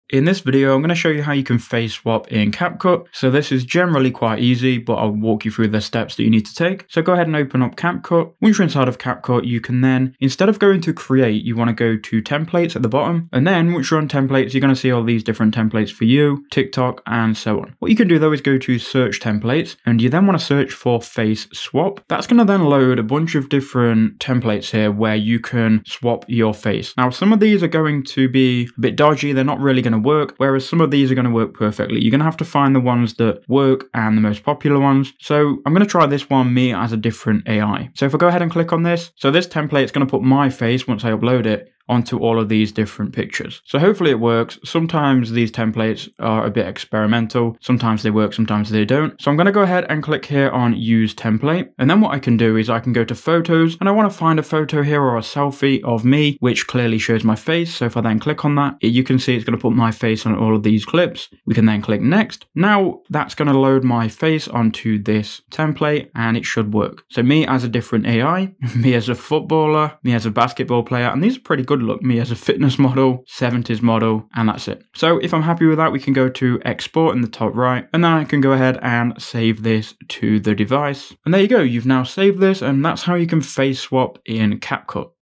How To Face Swap in Capcut - Quick Guide
In this video, I show you how to face swap in the Capcut app. This is a simple guide to using the face swap templates that you'll find on the Capcut app.